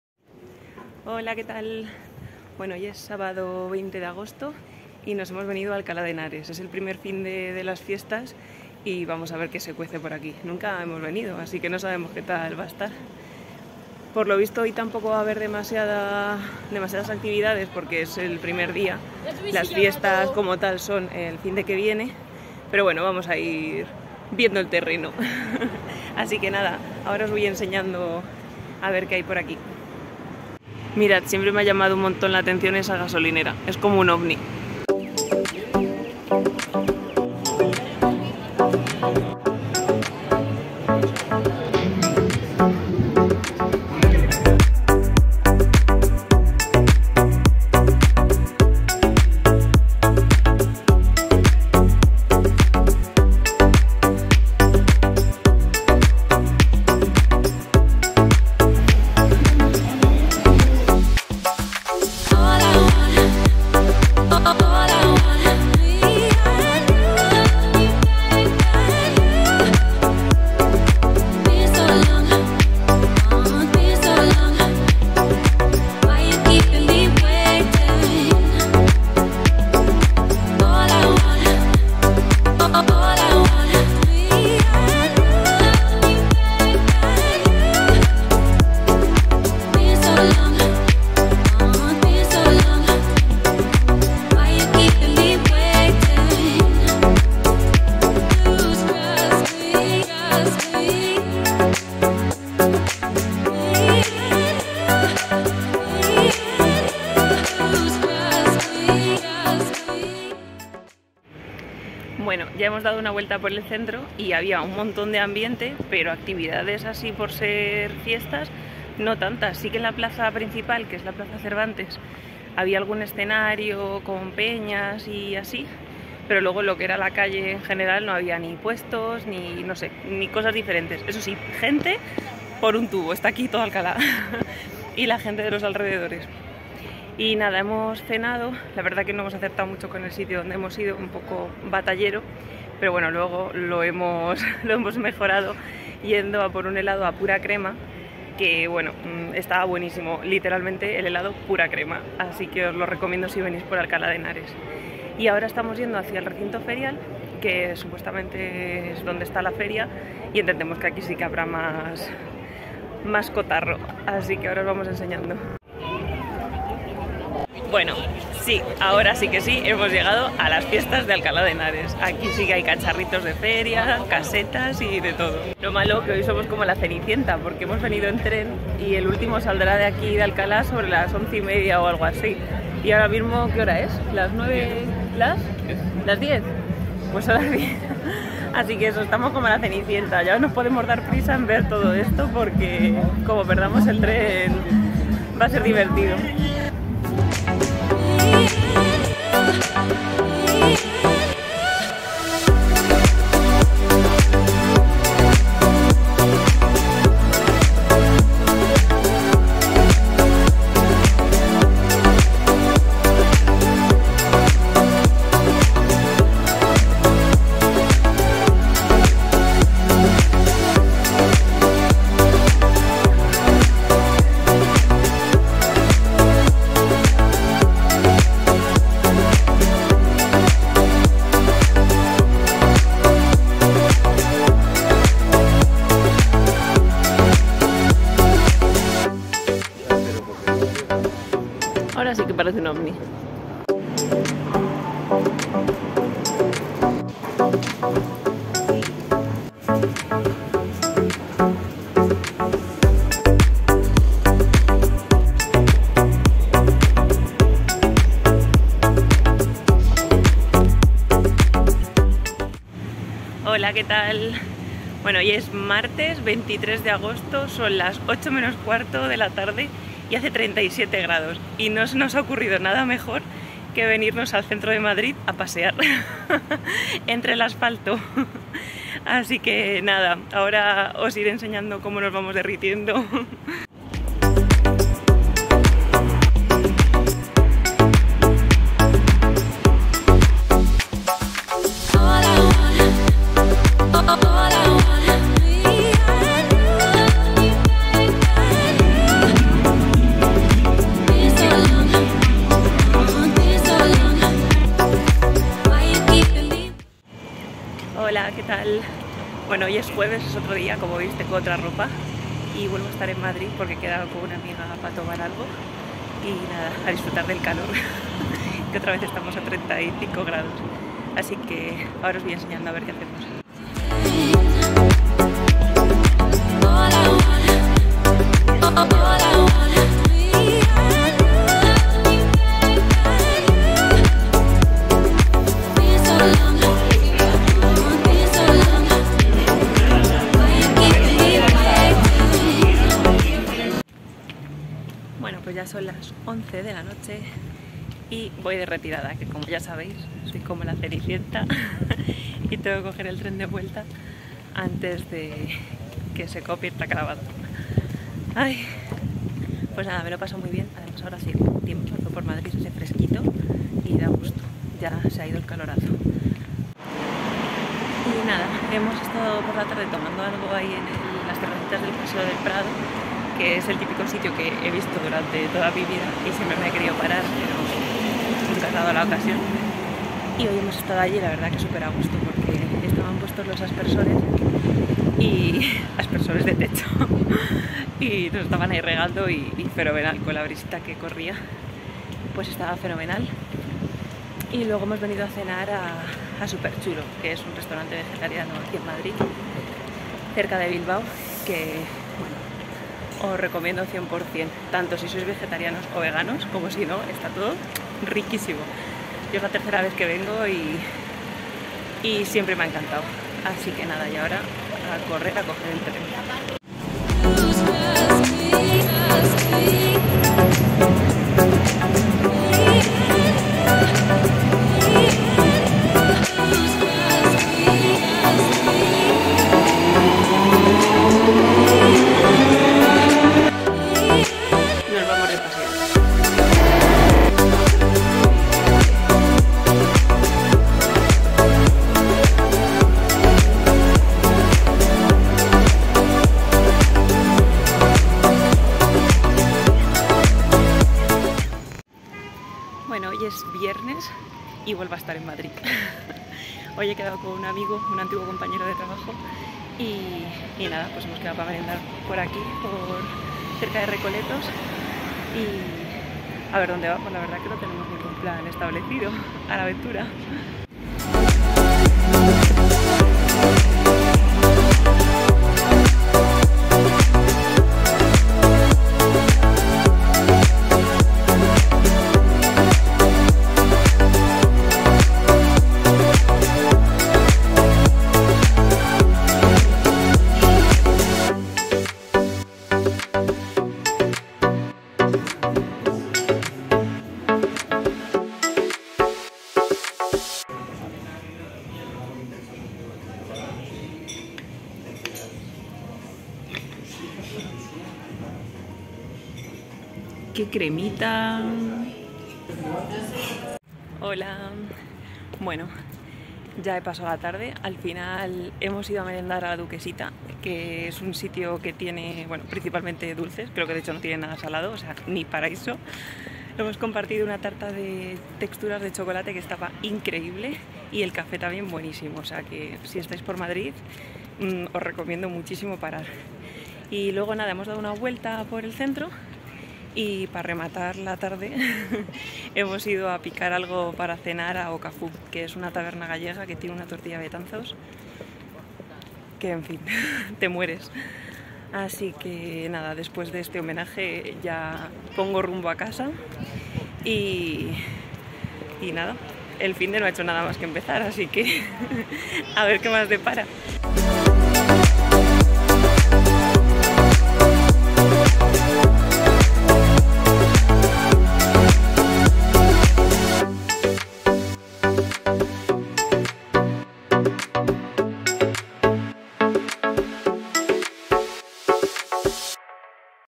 VLOG - DÍAS DE VERANO EN MADRID ☀🏙
Holaaaa! Hoy comparto con vosotros algunos de mis planes los últimos días de este verano en Madrid.

Como veis, no he parado! He estado en las fiestas de Alcalá de Henares y también he ido varios días por Madrid centro.